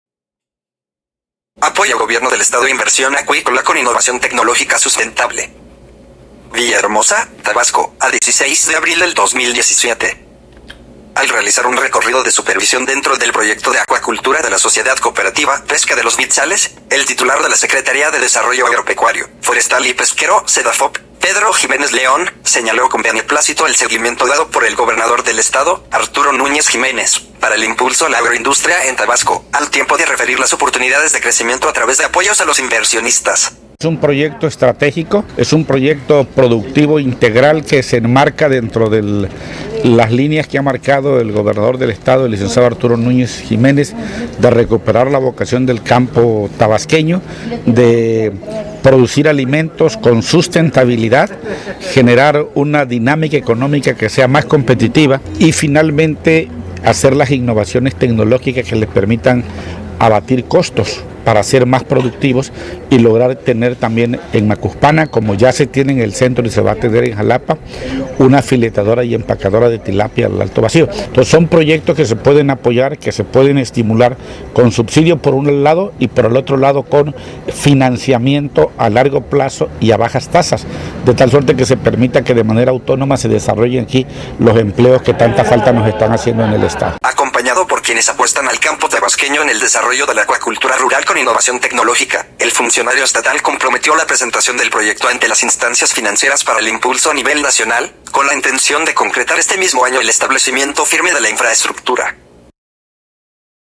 Apoya Gobierno del Estado inversión acuícola con innovación tecnológica sustentable
Villahermosa, Tabasco, a 16 de abril del 2017.

Al realizar un recorrido de supervisión dentro del proyecto de acuacultura de la sociedad cooperativa ‘Pesca de los Bitzales’, el titular de la Secretaría de Desarrollo Agropecuario, Forestal y Pesquero (Sedafop), Pedro Jiménez León, señaló con beneplácito el seguimiento dado por el gobernador del Estado, Arturo Núñez Jiménez, para el impulso a la agroindustria en Tabasco, al tiempo de referir las oportunidades de crecimiento a través de apoyos a los inversionistas.

Acompañado por quienes apuestan al campo tabasqueño en el desarrollo de la acuacultura rural con innovación tecnológica, el Funcionario Estatal comprometió la presentación del proyecto ante las instancias financieras para el impulso a nivel nacional, con la intención de concretar este mismo año el establecimiento firme de la infraestructura.